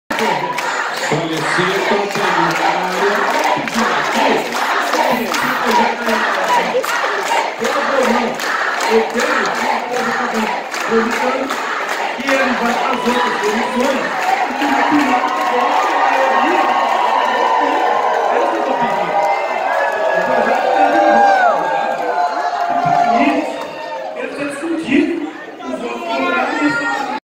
Protesto de professores e sindicato dentro da ALMS
O ato aconteceu na manhã de quarta-feira (11).
Tereré News.